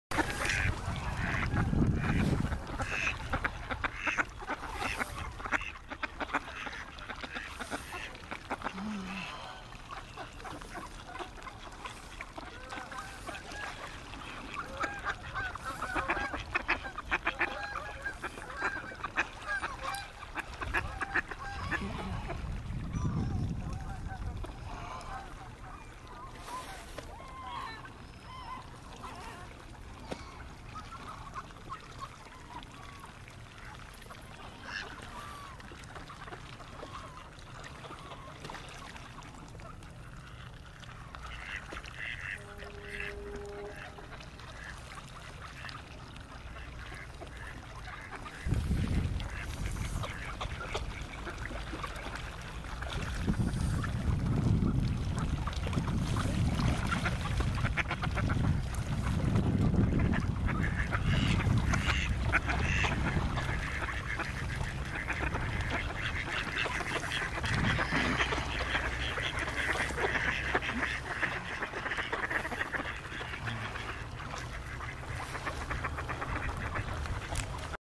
birds2020jan20p1
Waterfowls at the north shore of Lake Ontario in Toronto in the winter - mallards, buffleheads and seagulls.